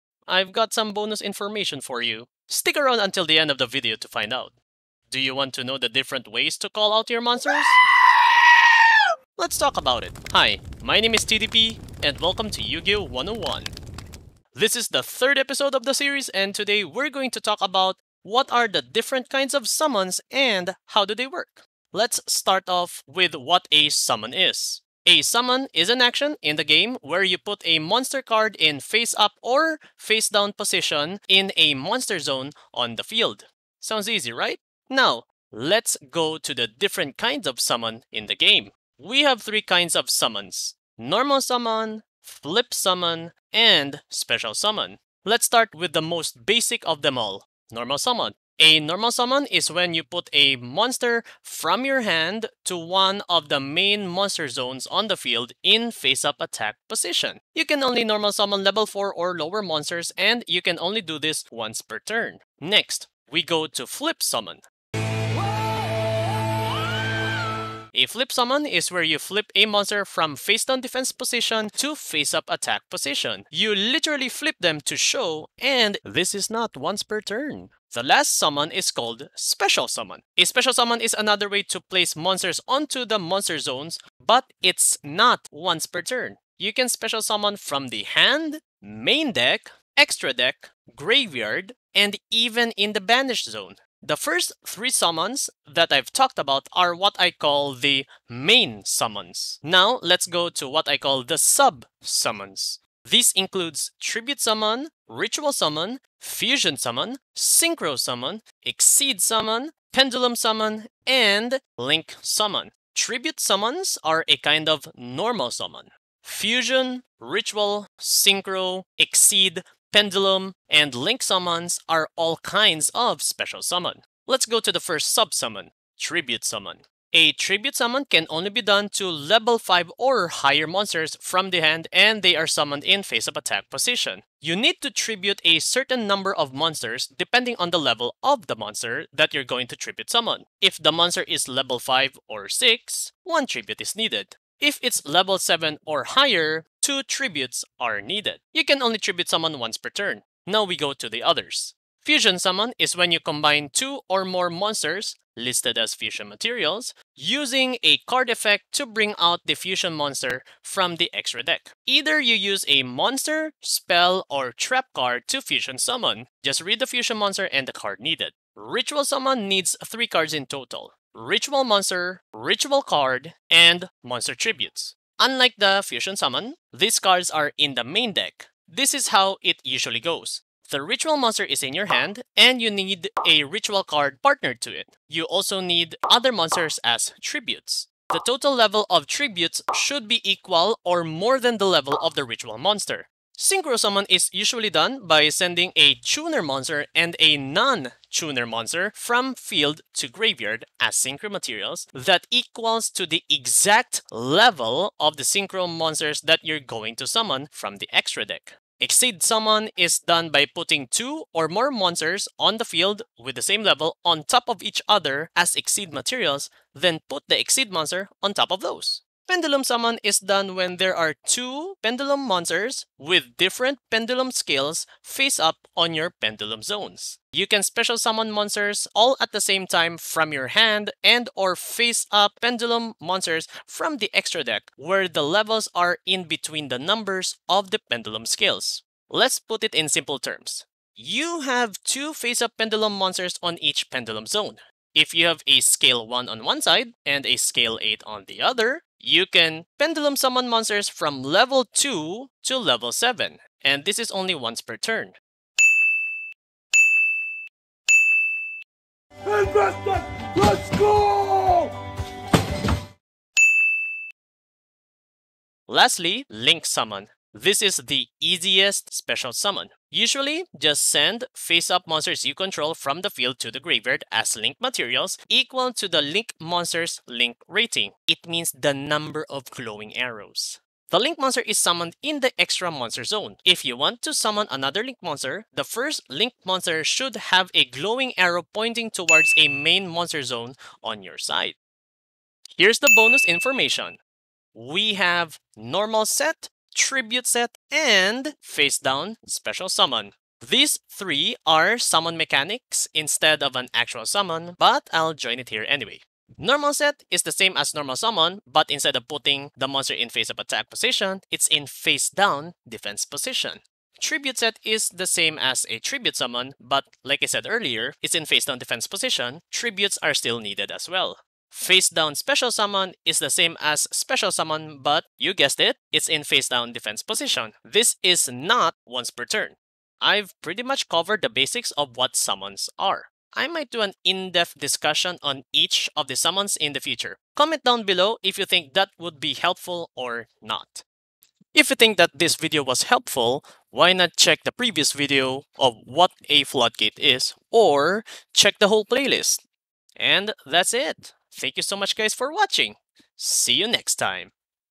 How to SUMMON in YUGIOH? | YUGIOH 101
Welcome to the third episode of the series. I'll be explaining what is a SUMMON and how it works.